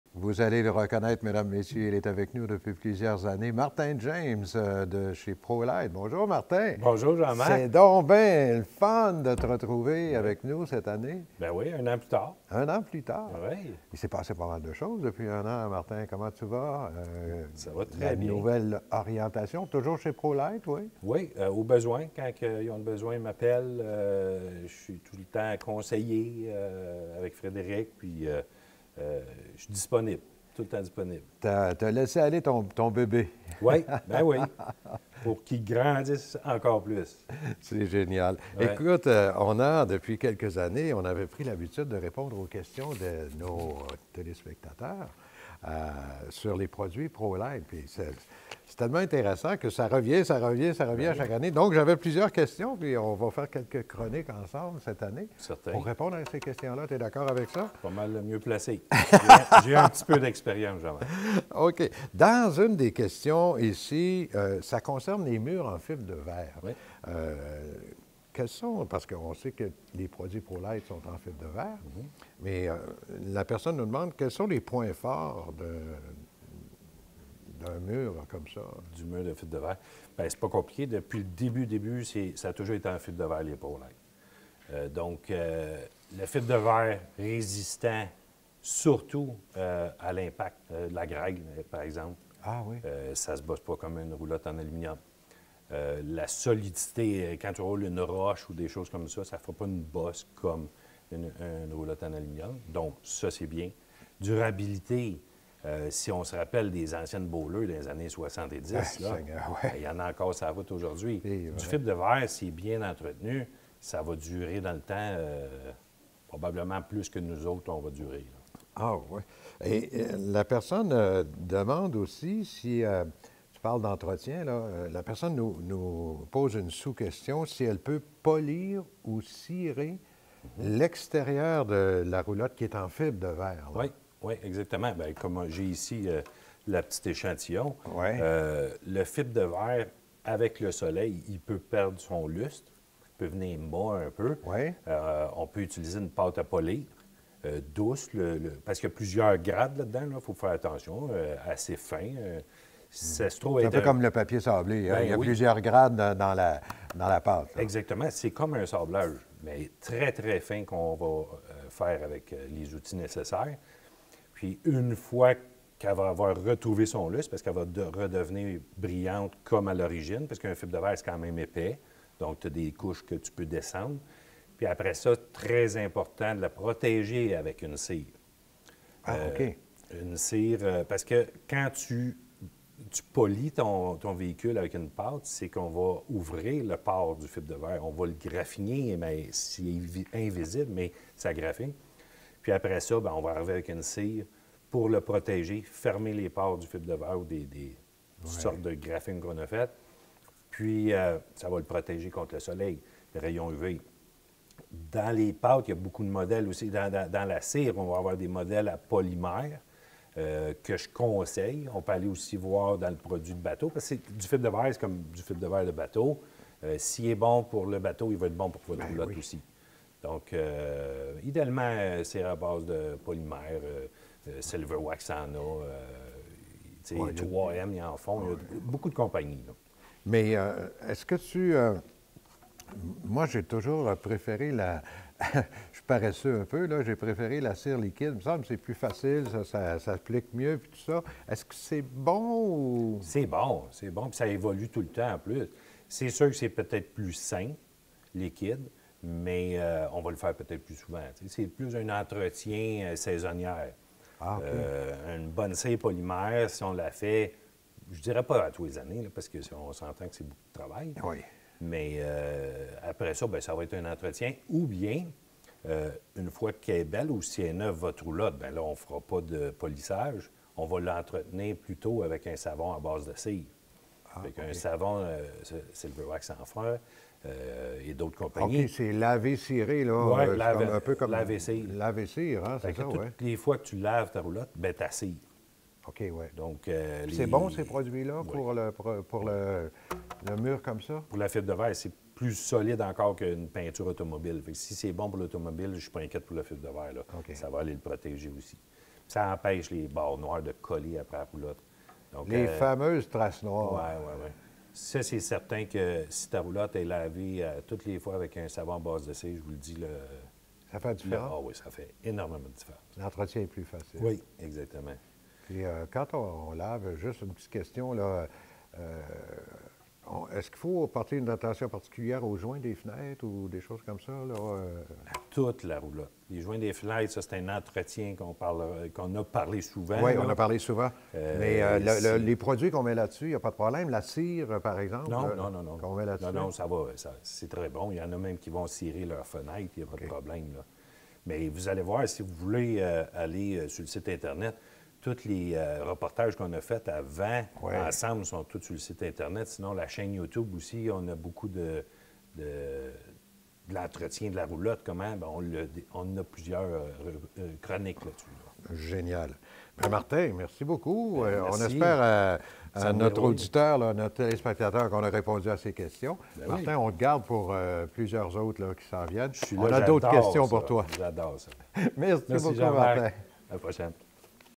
22 – Questions & Answers - Fibreglass | RV enthusiasts (English subtitles)
In this video Martin James, founder and former owner of Prolite, answers your questions about the fibreglass exterior of our trailers.